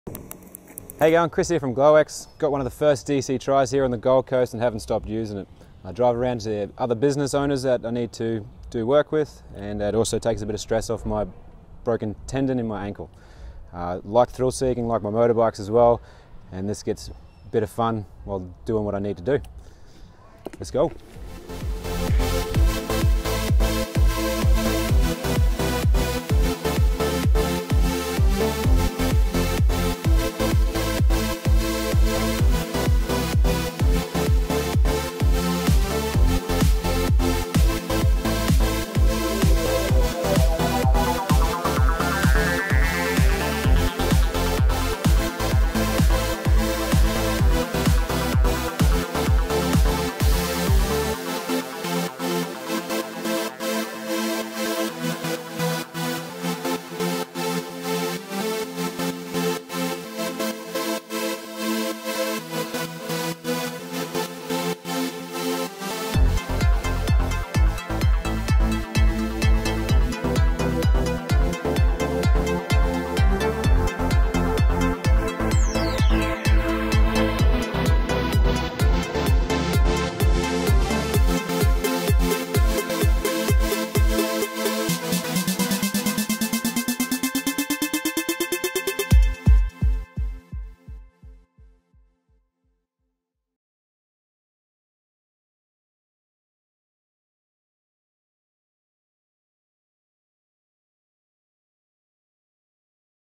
Kris From Glo-X loves his DC-Tri
Kris lets us know how he is enjoying his DC-Tri